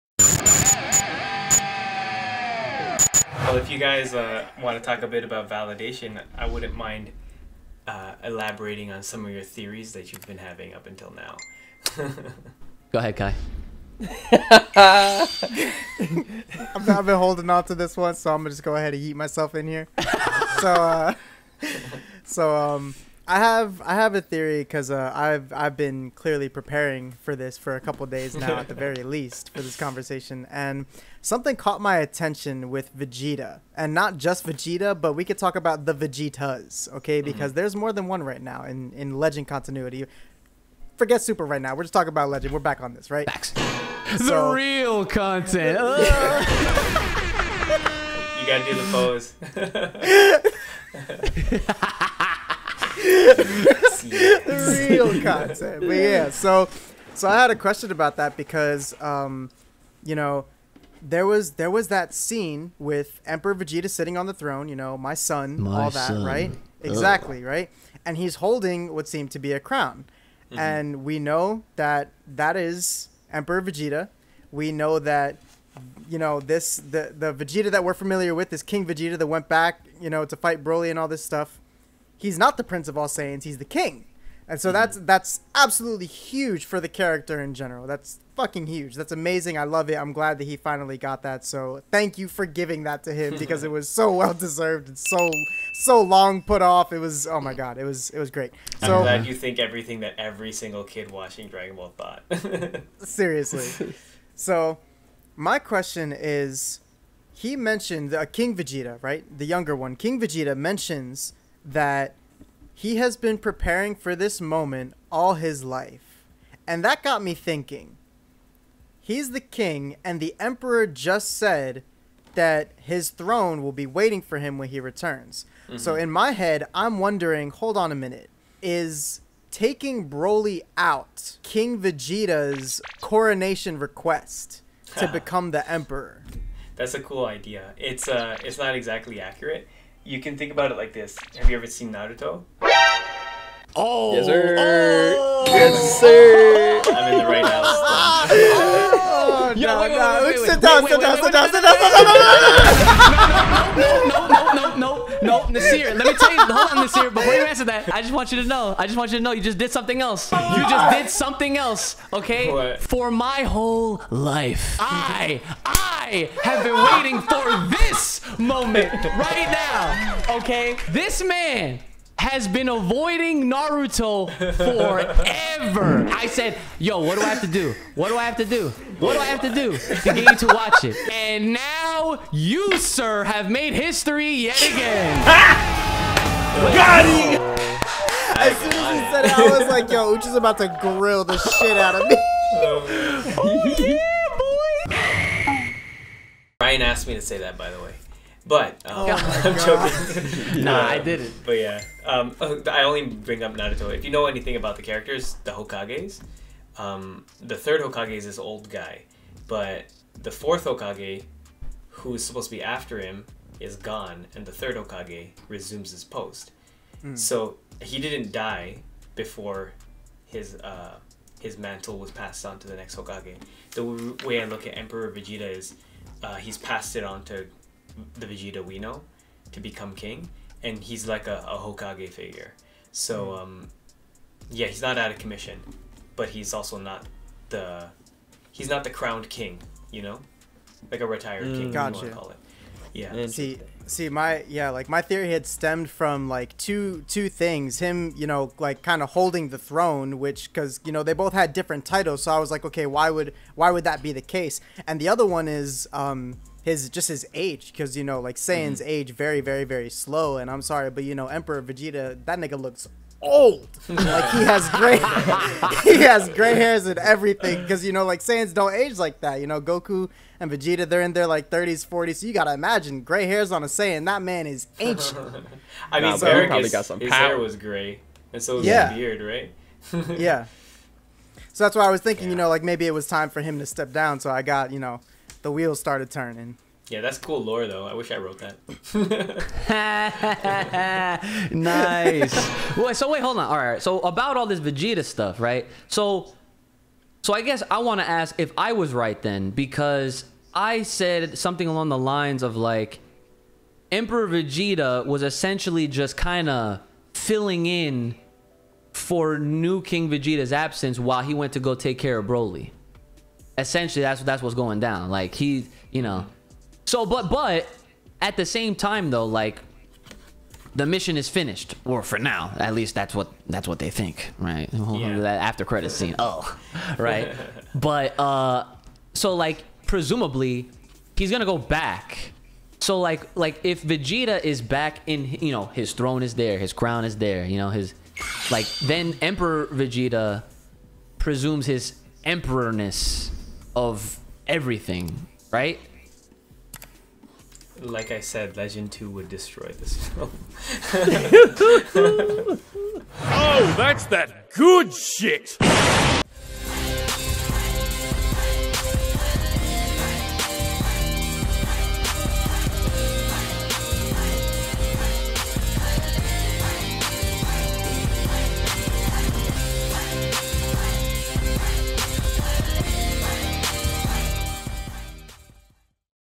Emperor and New King Vegeta EXPLAINED!! | Legend, A Dragon Ball Tale
What exactly are the roles that Emperor Vegeta and King Vegeta have? What is the relationship between the two?
Legend, A Dragon Ball Tale is single handedly the greatest fan project ever made that honestly holds it's own with delivering a unique, REIMAGINED world, consisting of familiar faces and concepts from the world of Dragon Ball. WE NEED THIS TO CONTINUE!!